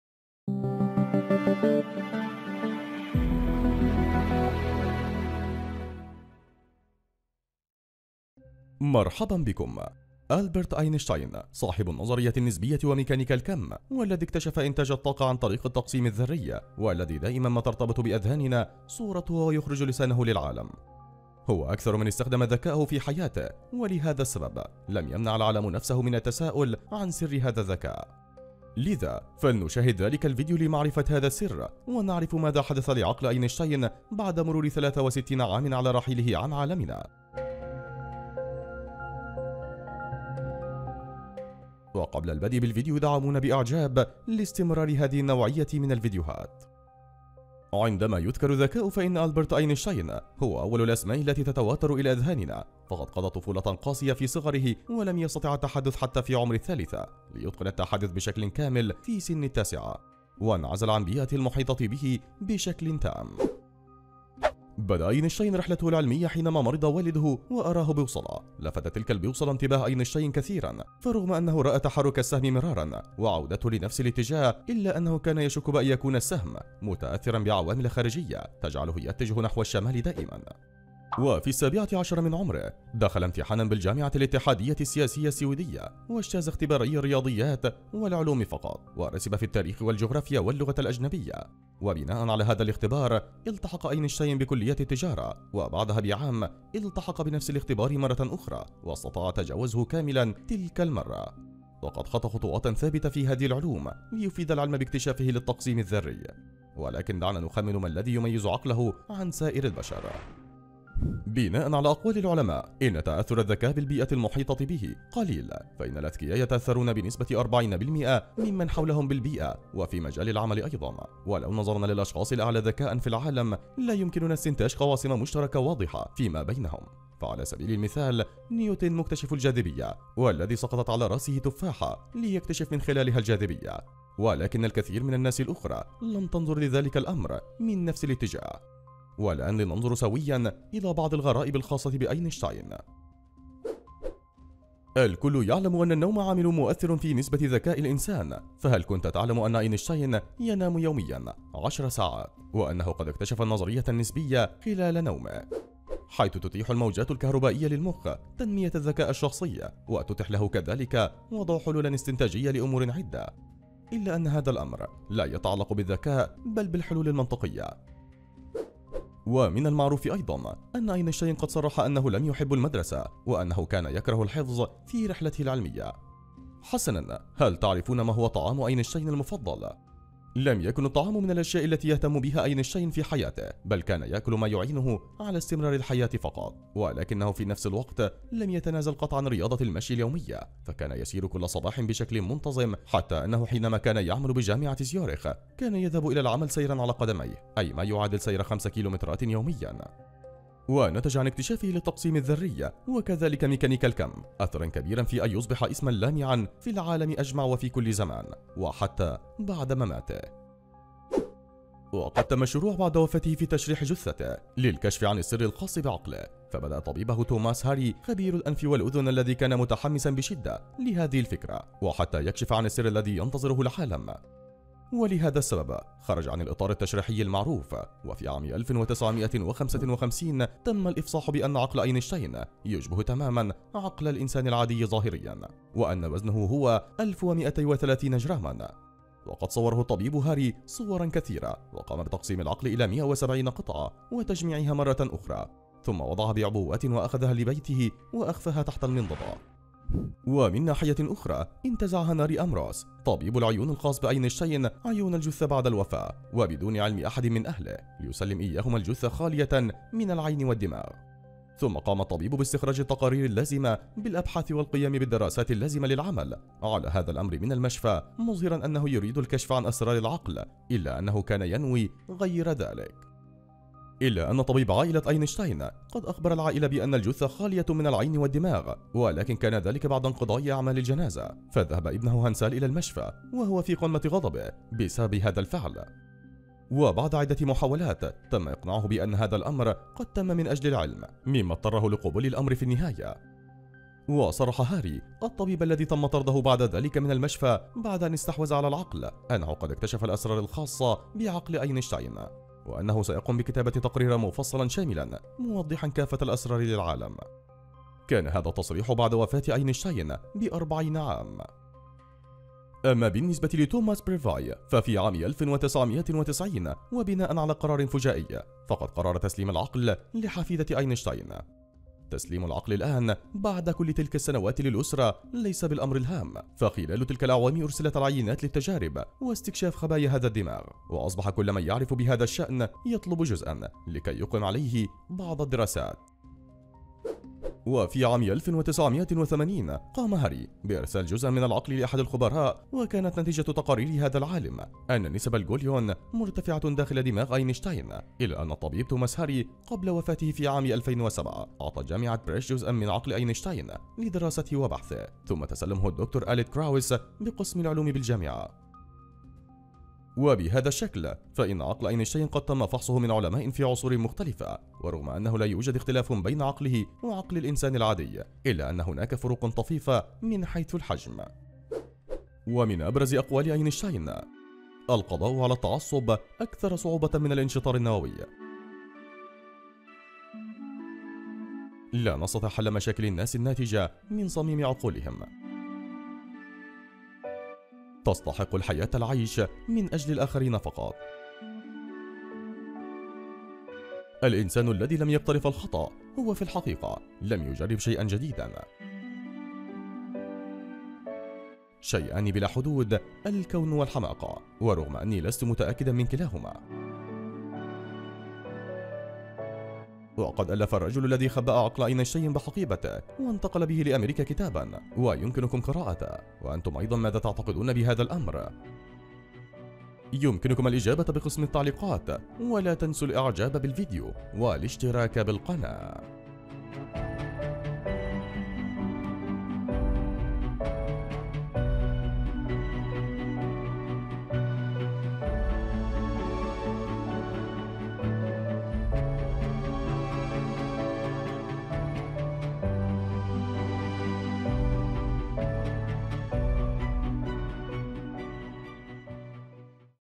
بعد 63 عام على وفاته.. هل تعلم ماذا حدث لمخ آينشتاين ؟ أسرار تعرفها لأول مرة !!
بعد 63 عام على وفاته.. هل تعلم ماذا حدث لمخ آينشتاين ؟ أسرار تعرفها لأول مرة .. ألبرت اينشاين صاحب نظرية النسبية ومكيانيكا الكم والذي إكتشف إنتاج الطاقة عن طريق التقسيم الذري. الذي دائماً ما يرتبط بأذهاننا صورته وهو يخرج لسانه للعالم. وهو أكثر من استخدم ذكائه في حياته ولهذا السبب لم يمنع العالم نفسه من التسائل عن سر هذا الذكاء! لذا فلنشاهد ذلك الفيديو لمعرفه هذا السر. ونعرف ماذا حدث لعقل آينشتاين بعد مرور 63 عام على رحيله عن عالمنا.